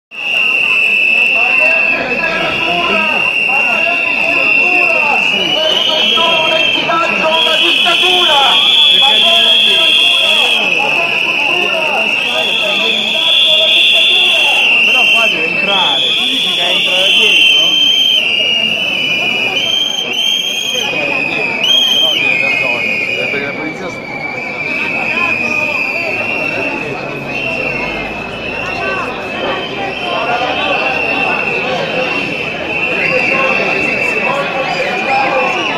Renzi contestato a Pescara
Fischi e urla a Pescara all'indirizzo del premier Matteo Renzi, in occasione dell'inaugurazione del Festival delle  letterature dell'Adriatico